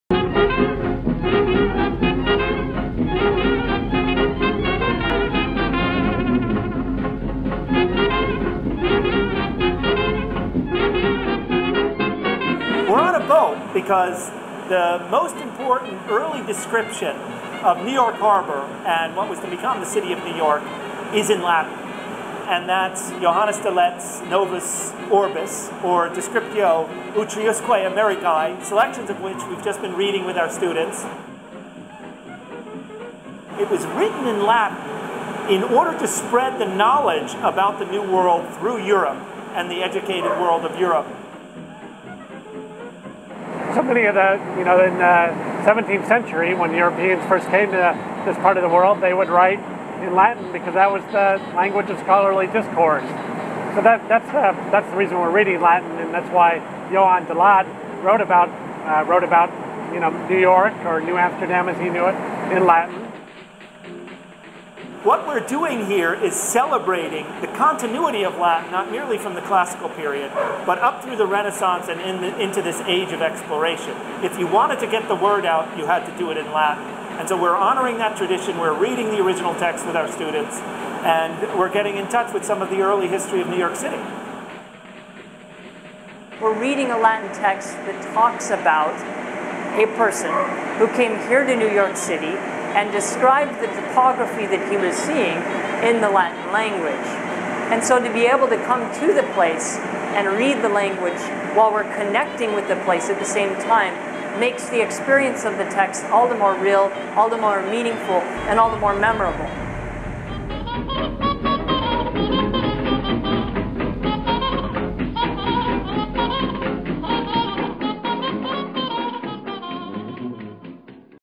A Boat Tour of New York City, In Latin
On September 22 the Fordham Classics Department took a boat trip around New York City and discussed early Latin texts about New York City.